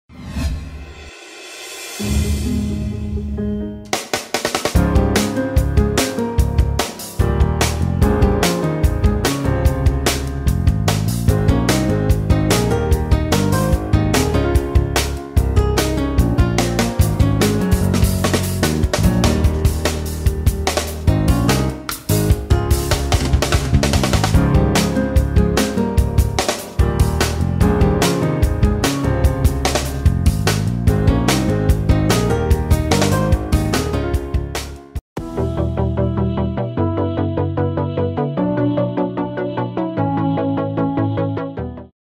Ganesh Motors || Two Wheelers Serving Centre || Kapra || ECIL || 9912060151 | zoneadds.com
Two wheeler servicing and repair @ Ganesh Motors, Kushaiguda. Have a look at their garage over here.